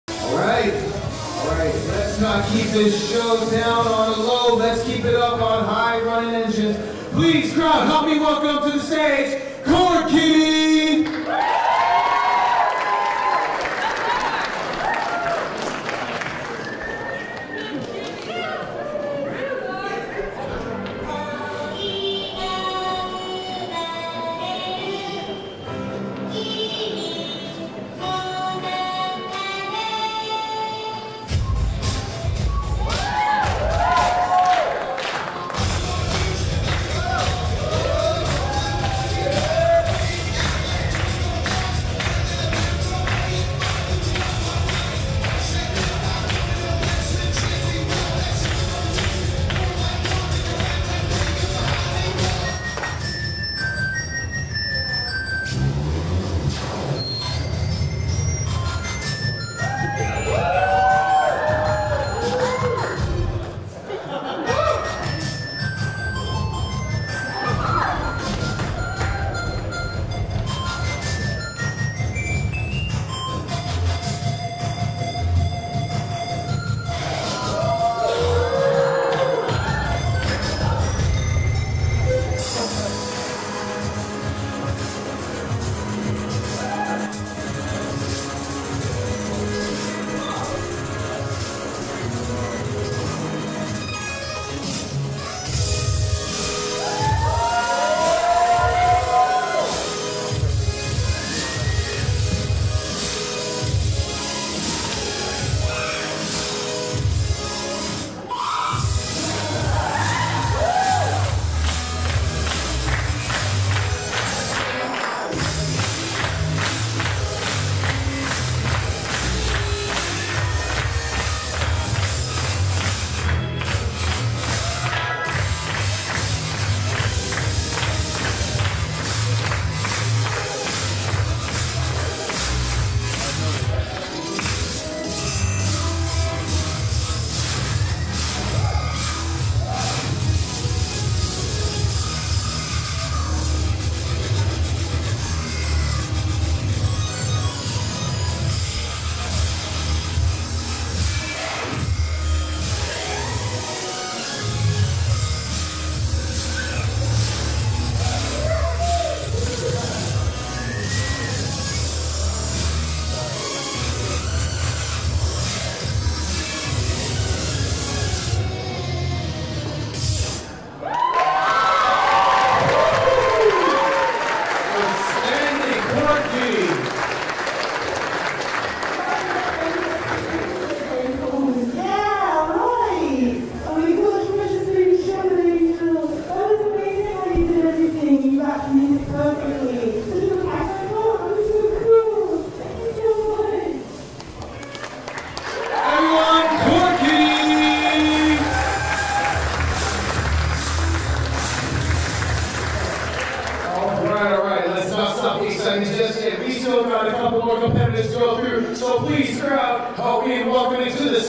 Furnal Equinox 2015 Dance Competition - Khord Kitty - Angle 2
The reason I reposted this video was because the official video is out of sync. The audio was posted 0.19 seconds behind the video.

As a result the dances end up looking off-the-beat. I fixed this on Adobe Premiere and posted this with corrected audio.

Enjoy! I had a lot of fun doing this dance.